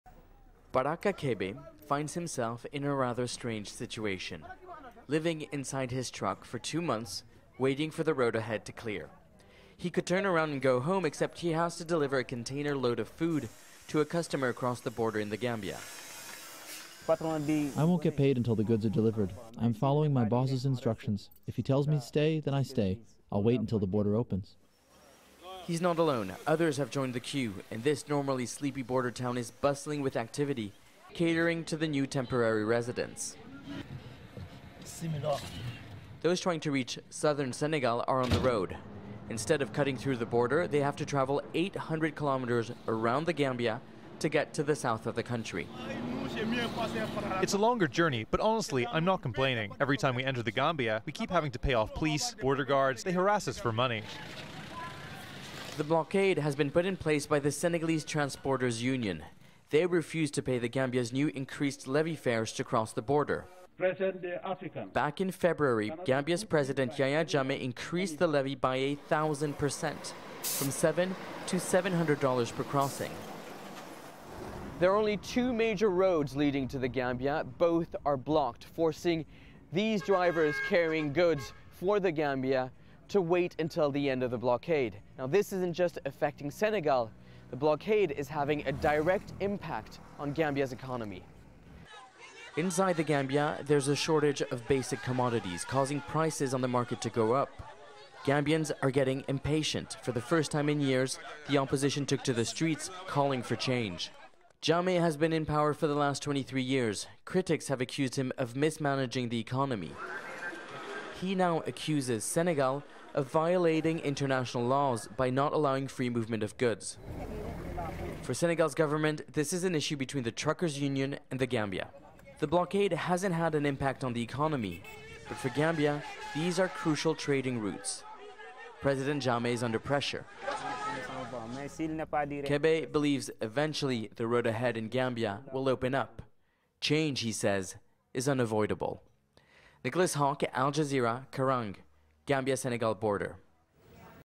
Gambia-Senegal border still closed over levy dispute
The border crossing between Senegal and Gambia has been shut for the last two months ever since Senegalese truck drivers refused to pay higher fees imposed by Gambia. This has led to massive traffic jam on the border. Gambia's president is accusing Senegal of breaking international law and is appealing for help from neighbouring countries.

Al Jazeera’s Nicolas Haque reports from Karrang at Gambia-Senegal borders.